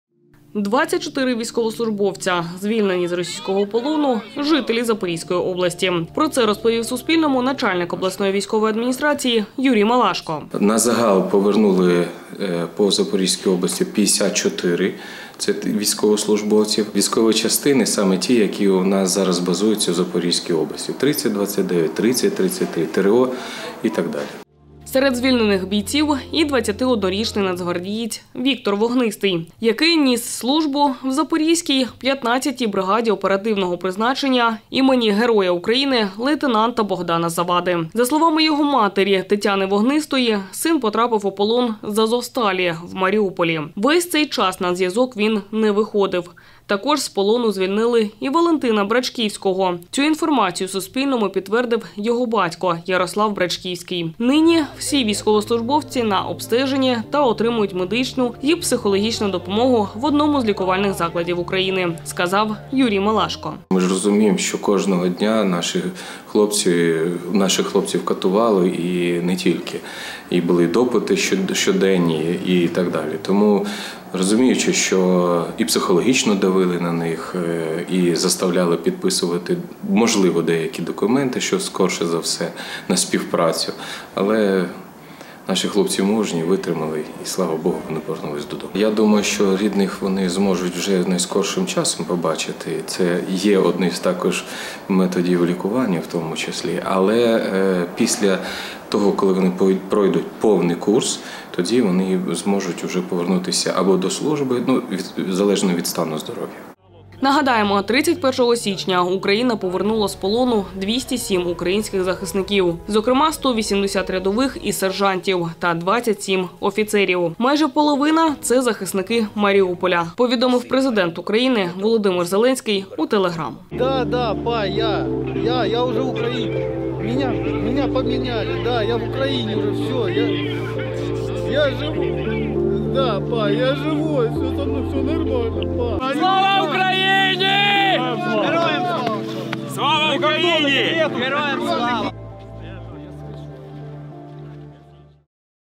З російського полону повернулося 24 жителя Запорізької області — Малашко
24 військовослужбовця, звільнені з російського полону, жителі Запорізької області. Про це розповів Суспільному начальник обласної військової адміністрації Юрій Малашко.